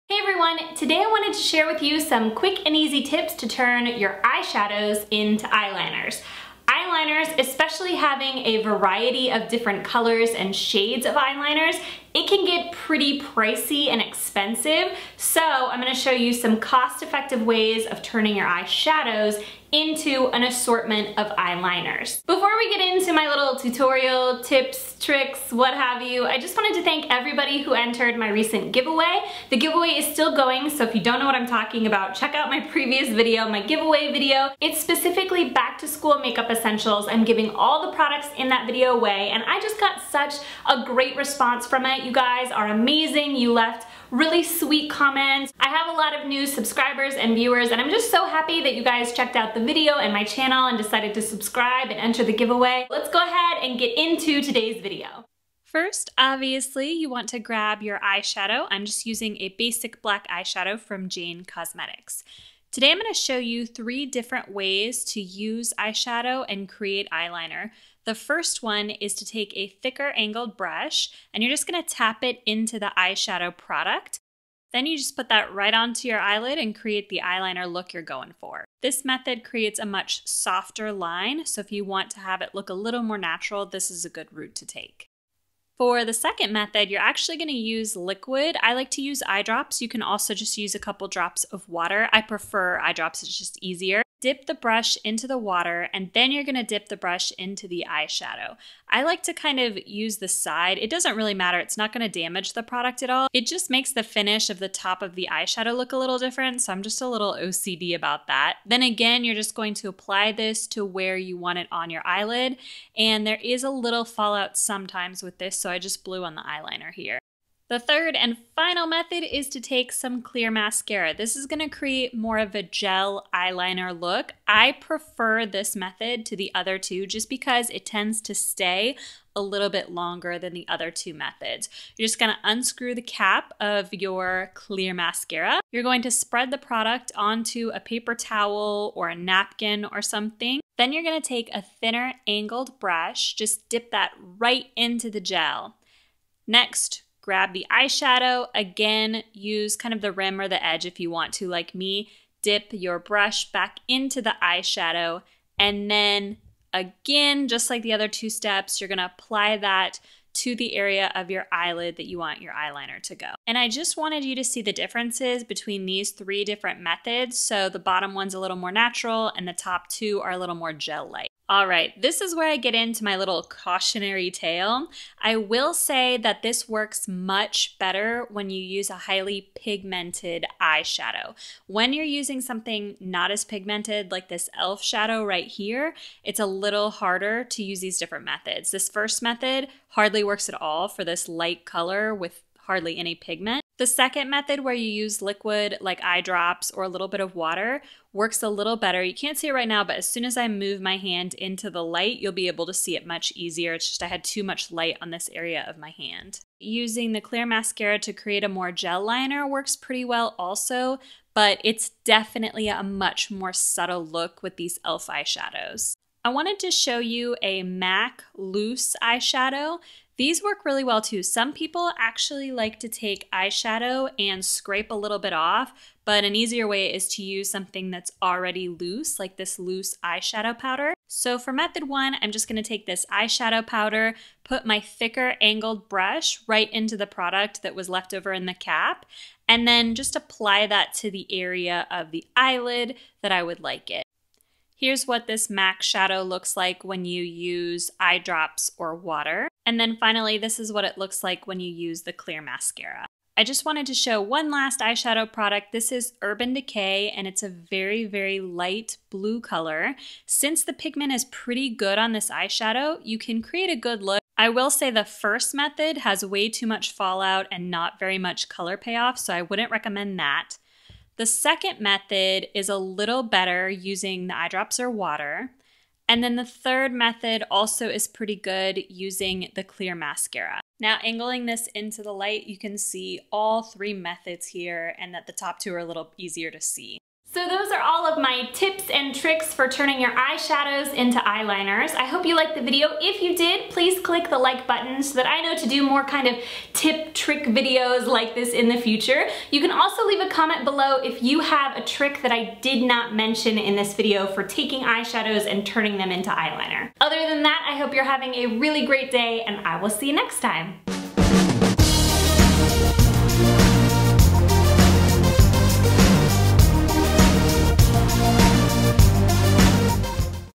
Turn Eye Shadow Into Eyeliner
Don’t want to spend a fortune on eyeliner?!? Use eye shadow to create your own eyeliner.

Makeup I'm Wearing:
-Makeup Forever High Definition Foundation in N117
-Tarte emphasEYES Waterproof Amazonian Clay Cream Eye Shadow in Slate
-Lorac Pro Palette in Champagne, Mauve, and Deep Purple
-Maybelline The Colossal Volum’ Express Waterproof Mascara in Glam Black
-e.l.f. Studio Contouring Blush & Bronzing Powder in St. Lucia